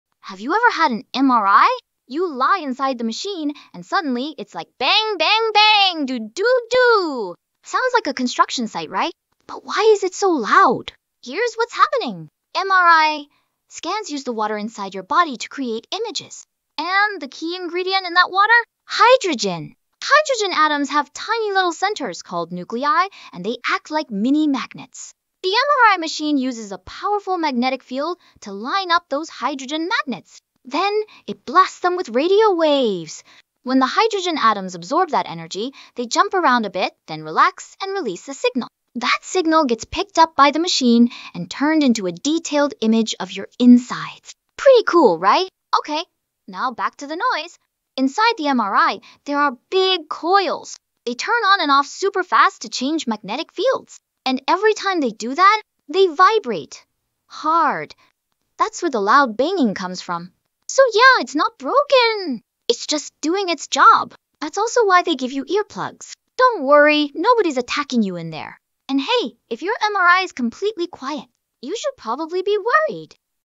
Why Is the MRI So Loud?!
Ever had an MRI and thought, “Why is this thing SO loud?”

This short video explains what’s really going on inside the machine —
and why it sounds like someone’s using a jackhammer next to your head.

From hydrogen atoms to magnetic coils,
here’s everything you need to know (in under a minute)!

🎧 Watch with headphones for the full experience!